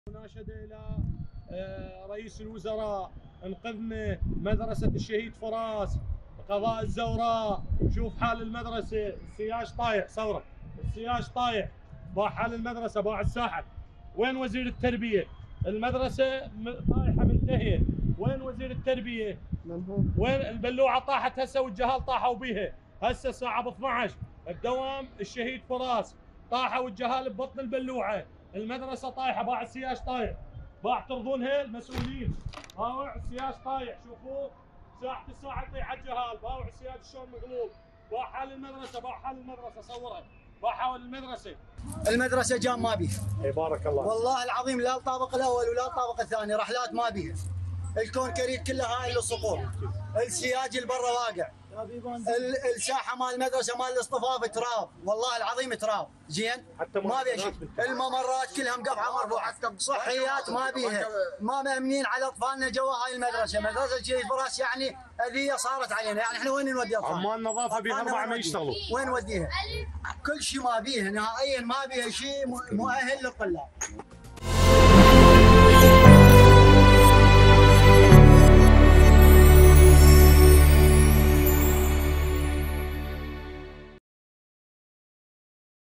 مواطنو قضاء الزوراء في بغداد يرصدون واقع المدارس في مناطقهم ويطالبون بحلول عاجلة
11258📺 افقي